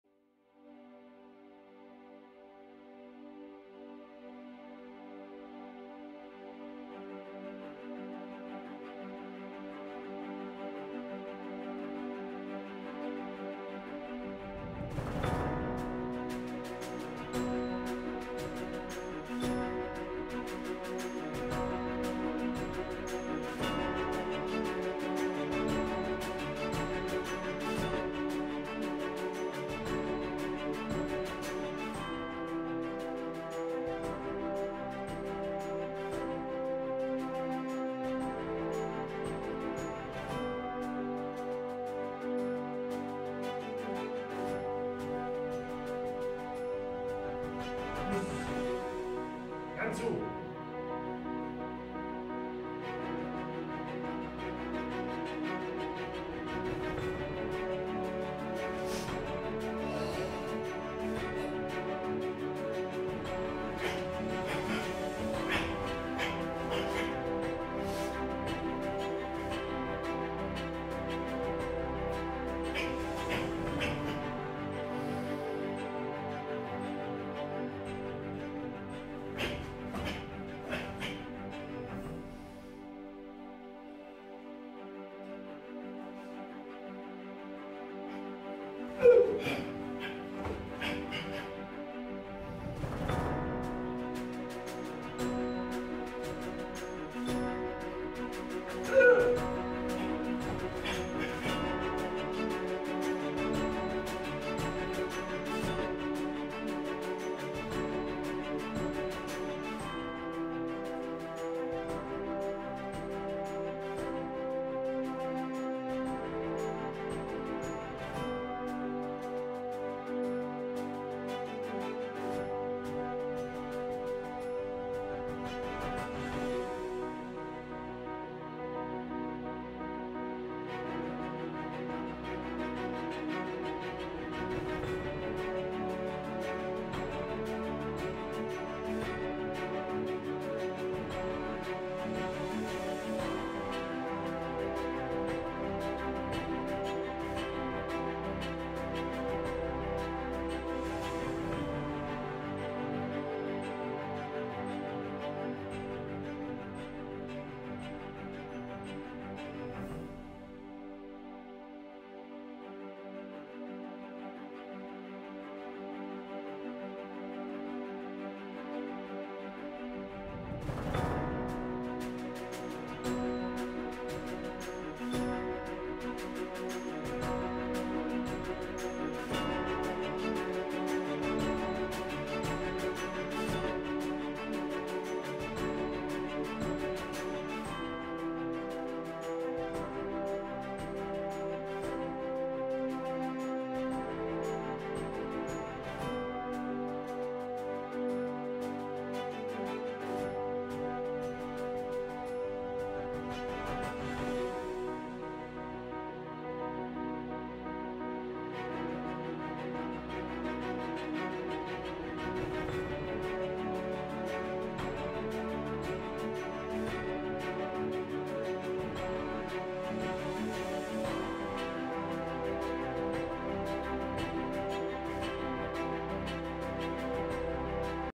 Kyokushin Kata - Yantsu
Die Kata "Yantsu" lässt sich sehr gut auf kleinsten Raum zuhause üben.